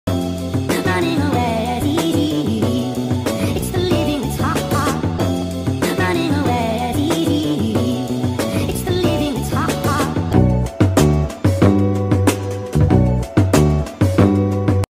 [as] [TW: P!LLS] [Happened last night]
!!PLEASE READ!!

Context:

Last night I was in a f!ght with my dad and I told him to shut up because he was screaming at me, because I said that he told me I wasn’t able to see some of my friends and that nearly pushed over the edge.

I was under a lot of stress already because I’m already failing some of my classes and we had to put Scooby down recently, so knowing I couldn’t see some of the people who calm me down and are basically my own therapy I almost did the unthinkable...

TW: Mention of SU!C!D£!!
Last night I had one of my p!ll b0ttles open in my hand contemplating on whether I should sw4ll0w the whole thing... or put it back, as you can probably guess I put it back, but in that minute... I thought my life was over... that I had nothing else to live for...
But then I thought of you all, my friends, my sister and mother!
I thought “why would I take there mother/friend/sister/daughter away...”
So I put them back and went back up stairs.

Please know I love you all with all my heart and you are part of the reason I’m still here...

Love,
      Kenzie/CG.